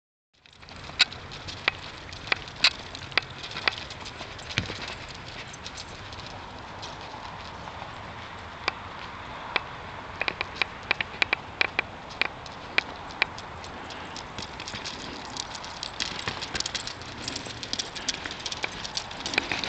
Croc Blanc 7
avec une Crazy ball sur la patinoir de mon quarcier